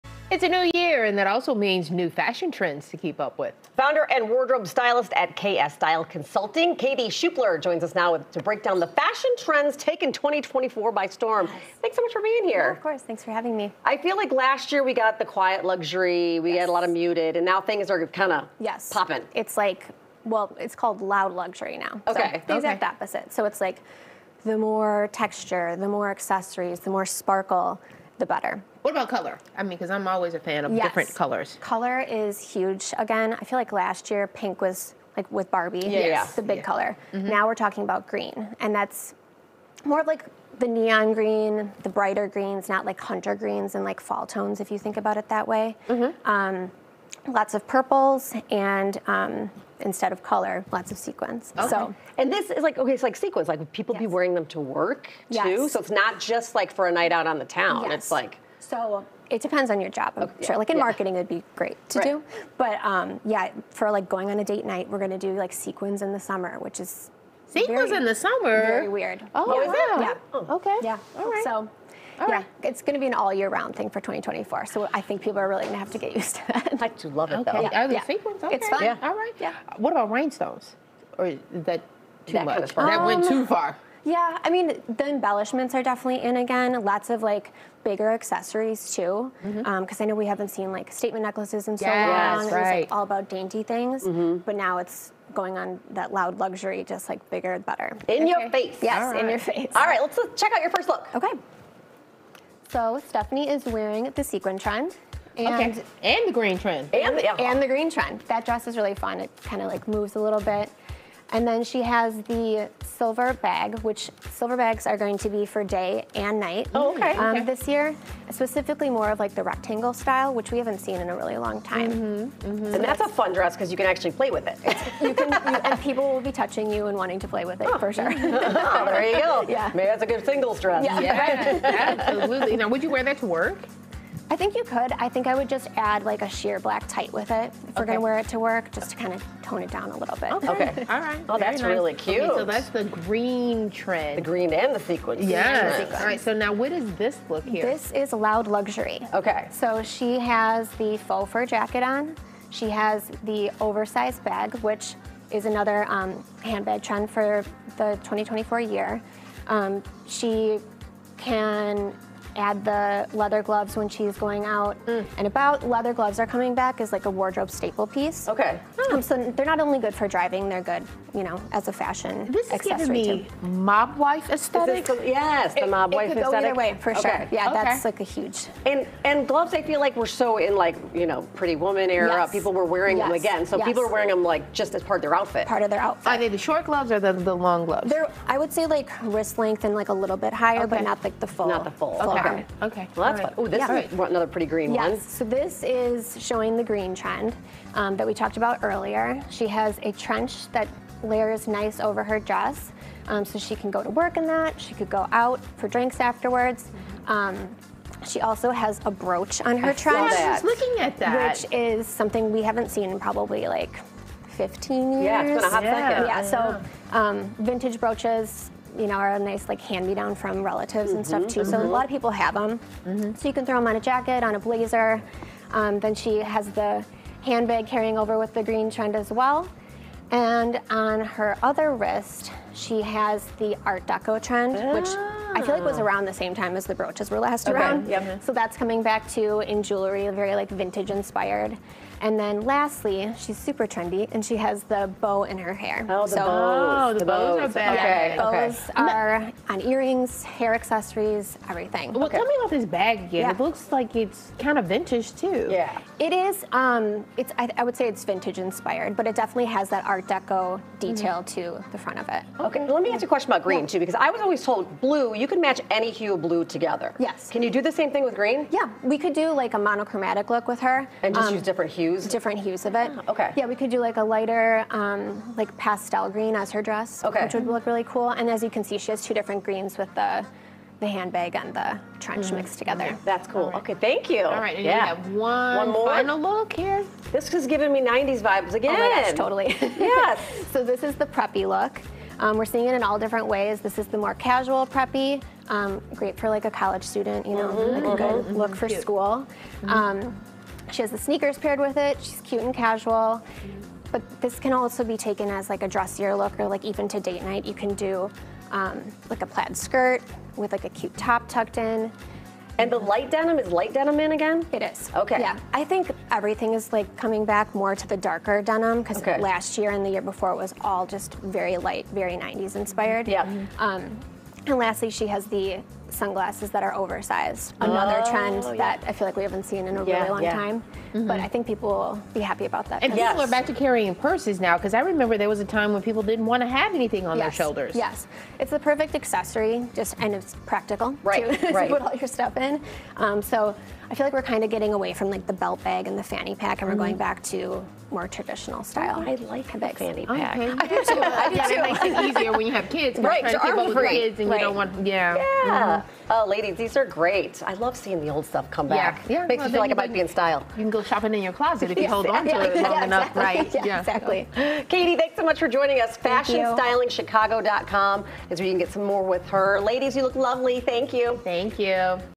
The Hottest Fashion Trends of 2024
It's a new year and that also means new fashion trends to keep up with.

Founder and wardrobe stylist at KS Style Consulting, Katie Schuppler broke down the fashion trends taking 2024 by storm.